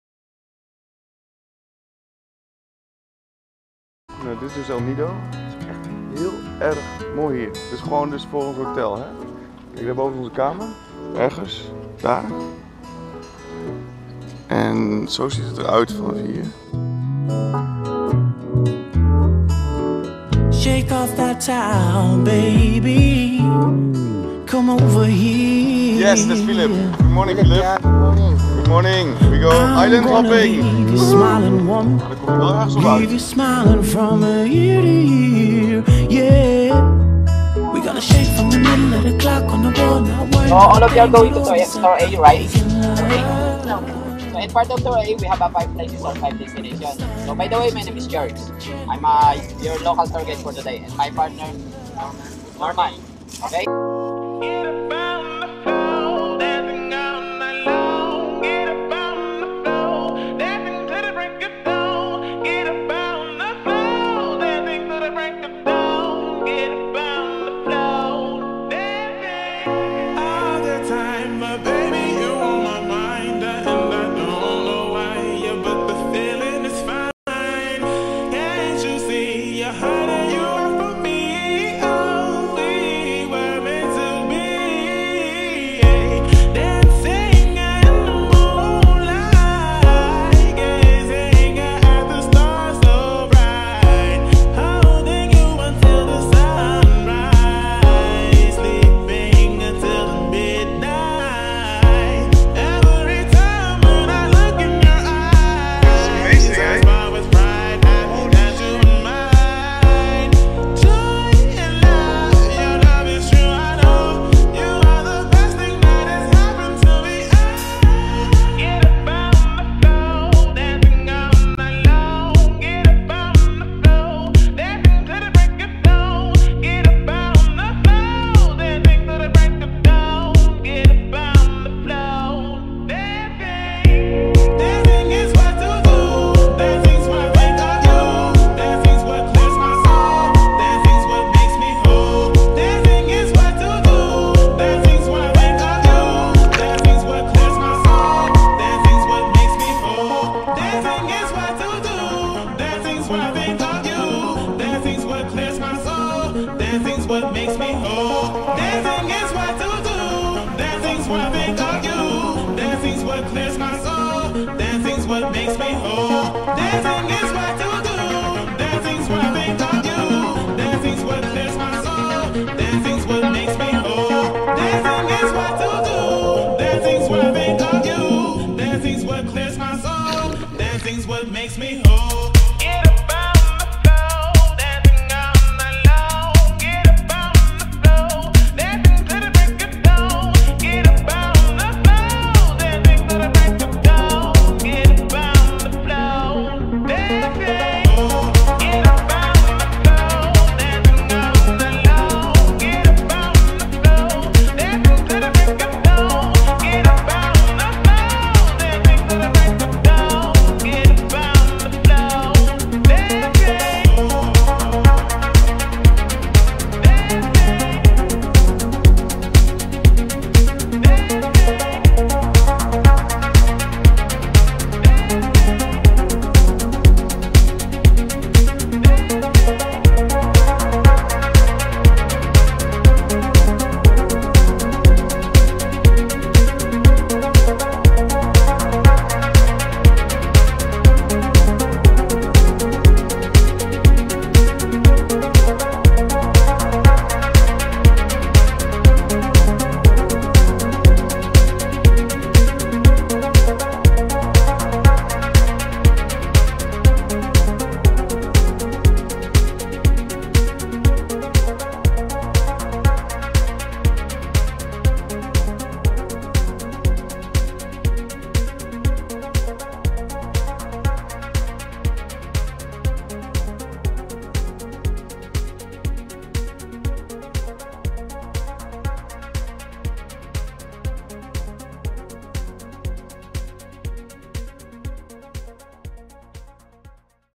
VLOG #2: ISLAND TOUR A, EL NIDO, PHILIPPINES - MICHIEL&CLAUDIA
Onze tweede VLOG gaat over tour A die wij deden in El Nido op de Filipijnen in januari! Het is daar echt prachtig, echt een aanrader!
Our second VLOG is an edited clip from our trip to El Nido in the Philippines. There we did island hopping tour A on January 29, 2016 .  Amazing scenery and experience! Hope you enjoy it!